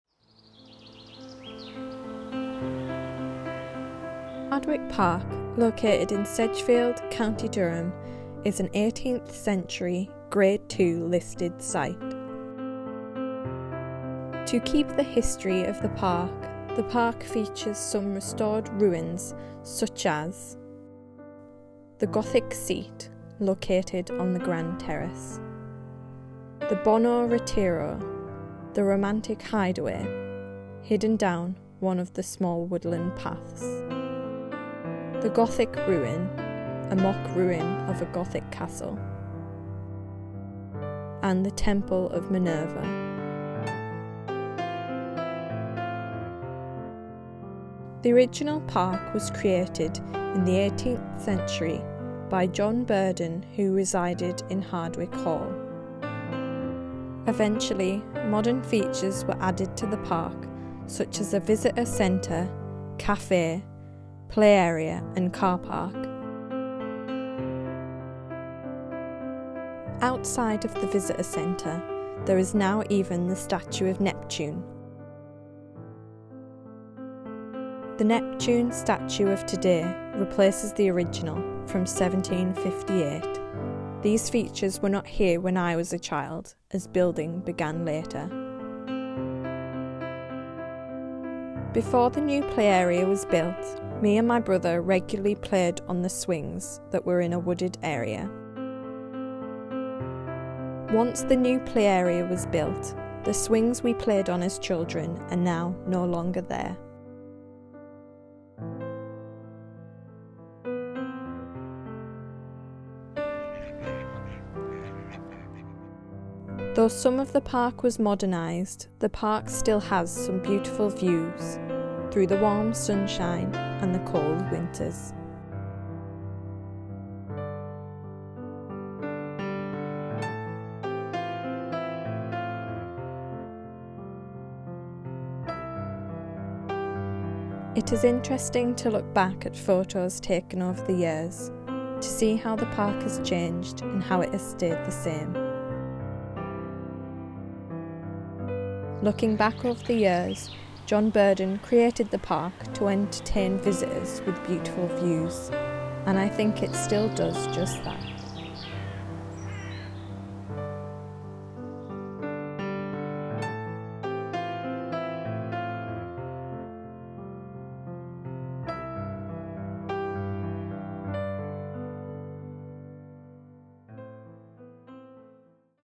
Hardwick Park | Short Film
A short film I created about a park that I have visited for years. This is the first video I've ever filmed and edited and includes some history of the park, popular park features, and photos I have taken over the years.

Filmed at Hardwick Park, County Durham.